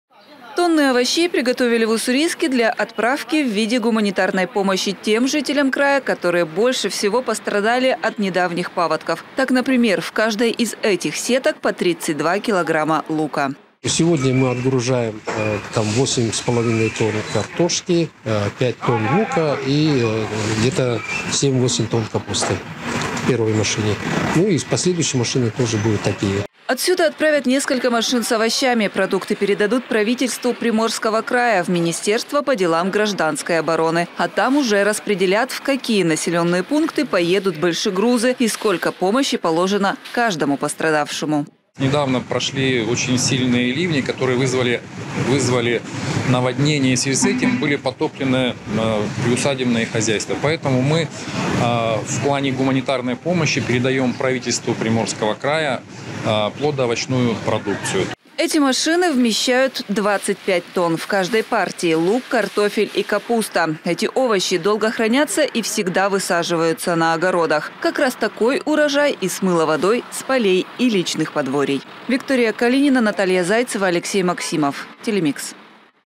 Помощь пострадавшим. Груз овощей отправился в подтопленные регионы края
Несколько большегрузов с овощами отправились из Уссурийска по краю. Это гуманитарная помощь, которую распределят по территориям, пострадавшим от наводнения в августе и сентябре.